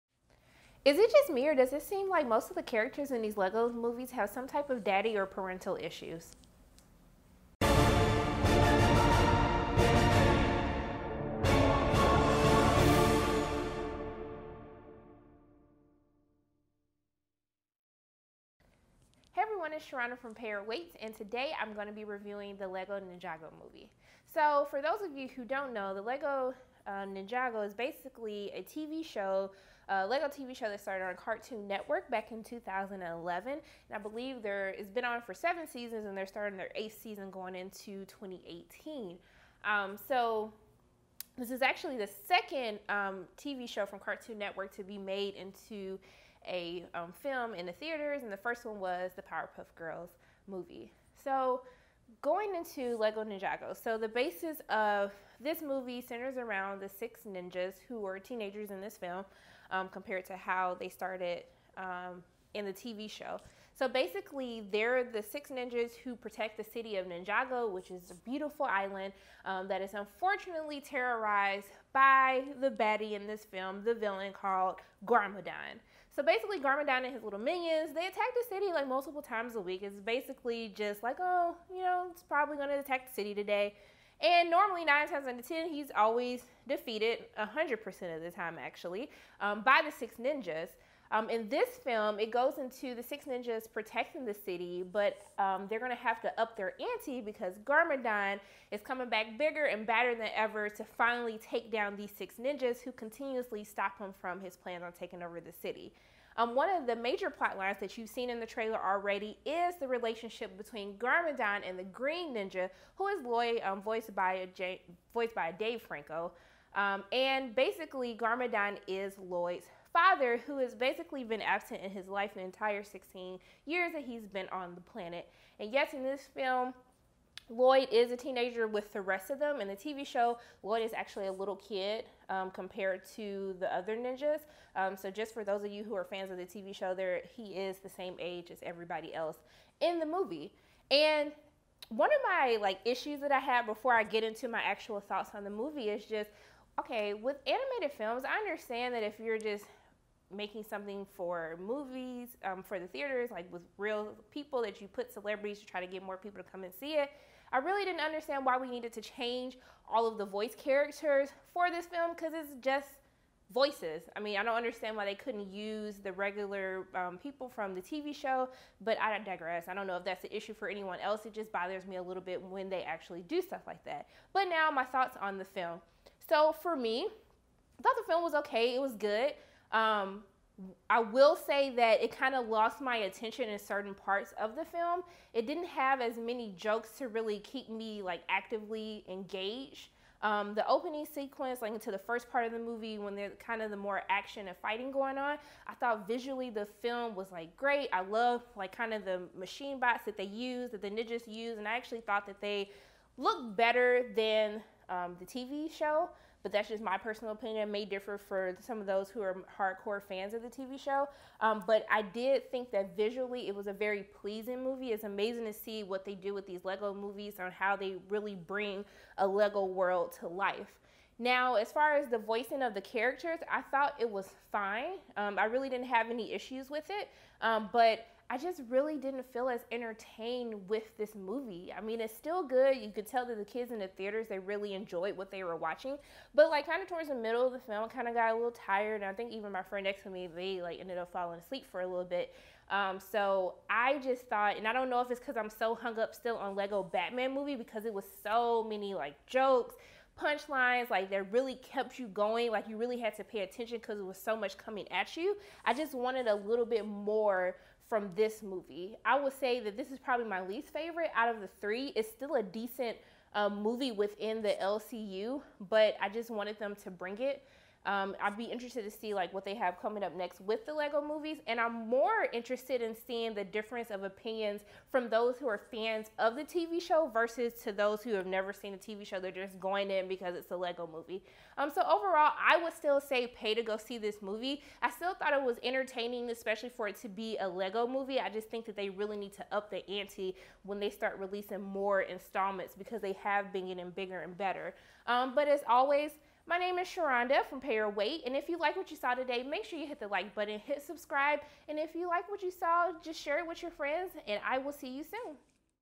The Lego Ninjago Movie Review
My thoughts on The Lego Ninjago Movie out in theaters this Friday.